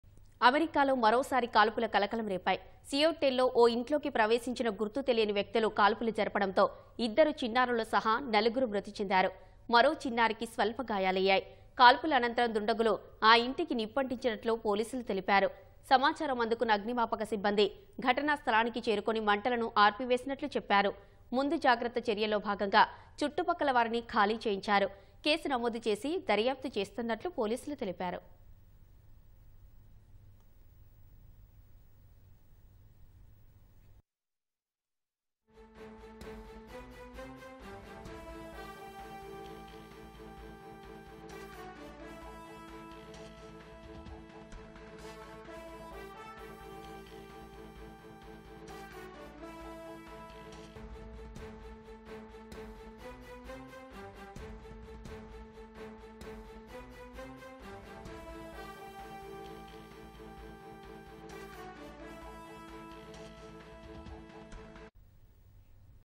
4 People Dead | US Mass Shooting | అమెరికాలో మరోసారి కాల్పులు కలకలం | నలుగురు మృతి
అమెరికాలో మరోసారి కాల్పులు కలకలం రేపాయి. సియోటెల్ లో ఓ ఇంట్లోకి ప్రవేశించిన గుర్తు తెలియని వ్యక్తులు కాల్పులు జరపడంతో  ఇద్దరు చిన్నారులు సహా నలుగురు మృతి చెందారు. మరో చిన్నారికి స్వల్ప గాయాలయ్యాయి. కాల్పుల అనంతరం దుండగులు  ఆ ఇంటికి నిప్పంటించినట్లు  పోలీసులు తెలిపారు. సమాచారం అందుకున్న అగ్నిమాపక సిబ్బంది...ఘటనాస్థలానికి చేరుకొని  మంటలను ఆర్పివేసినట్లు చెప్పారు. ముందుజాగ్రత్త చర్యల్లో భాగంగా చుట్టుపక్కల వారిని ఖాళీ చేయించారు. కేసు నమోదు చేసి...దర్యాప్తు చేస్తున్నట్లు పోలీసులు తెలిపారు.